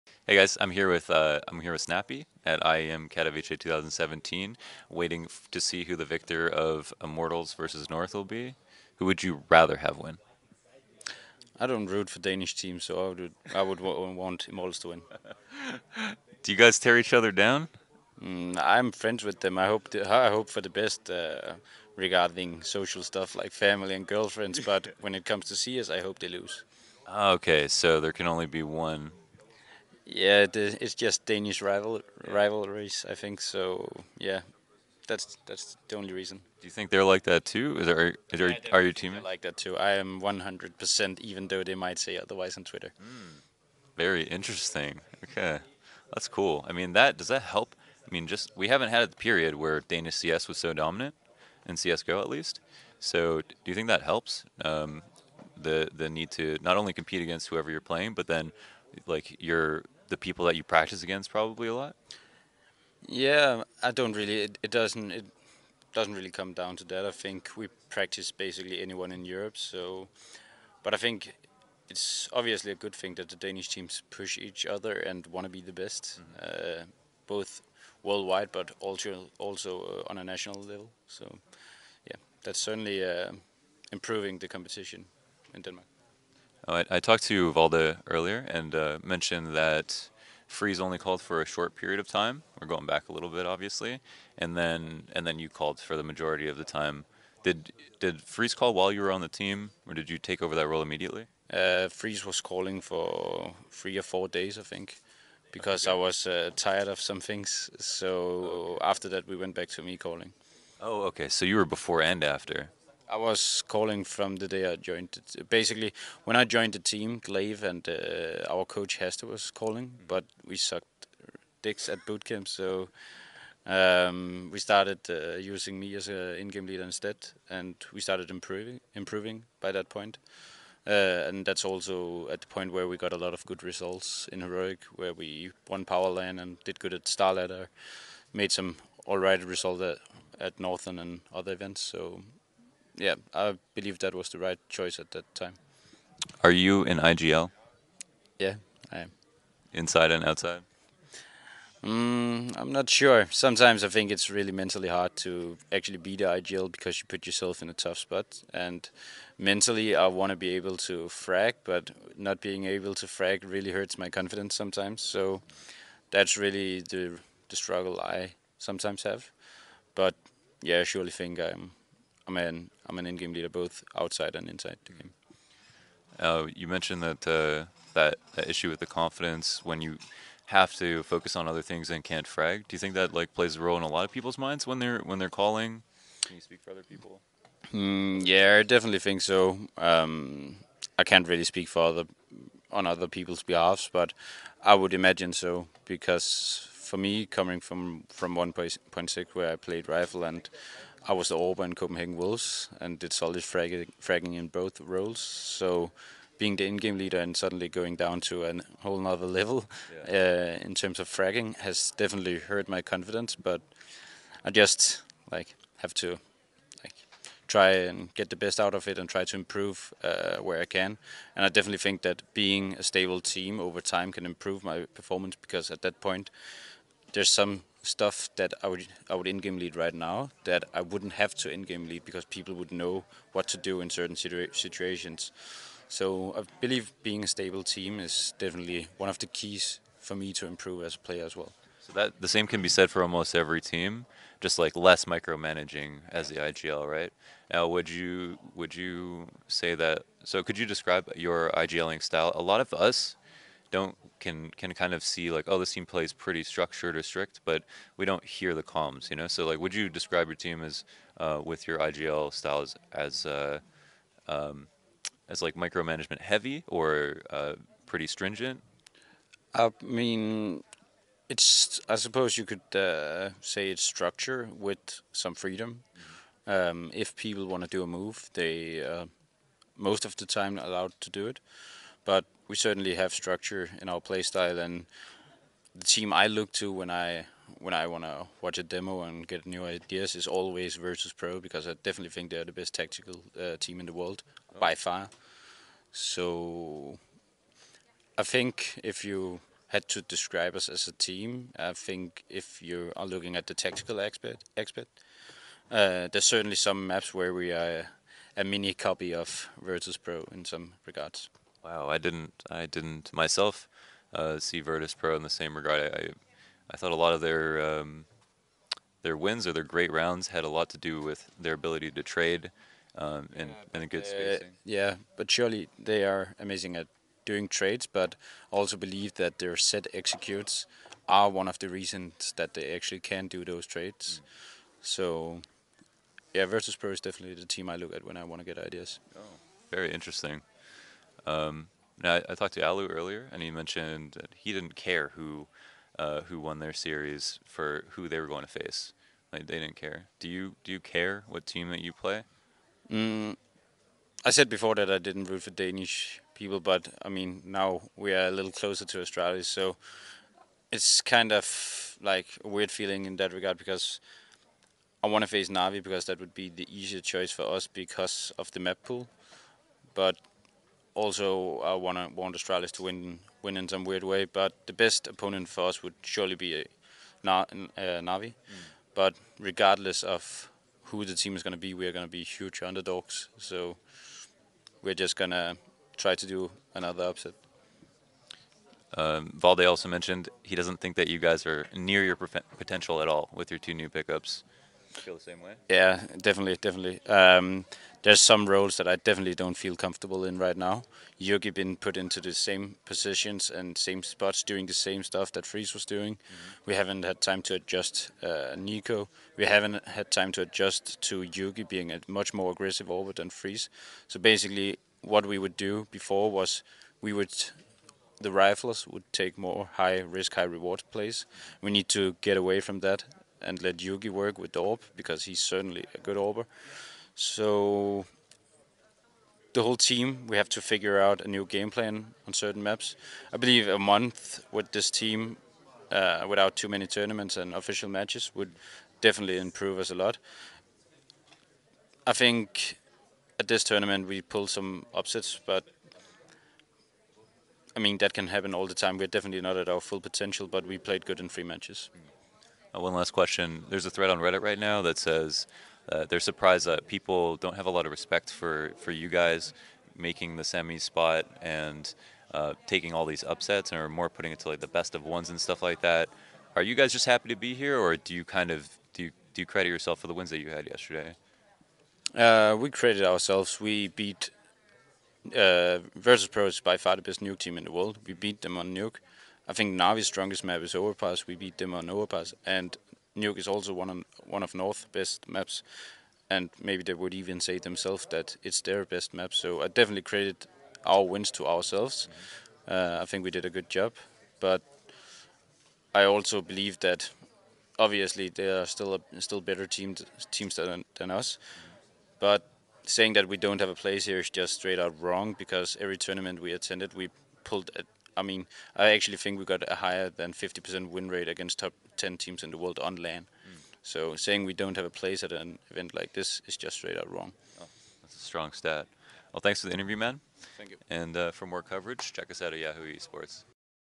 Heroic Snappi on being an IGL: ‘Not being able to frag really hurts my confidence’
Marco "Snappi" Pfeiffer breaks down fragging as an in game leader and Heroic's surprise performance at IEM Katowice 2017.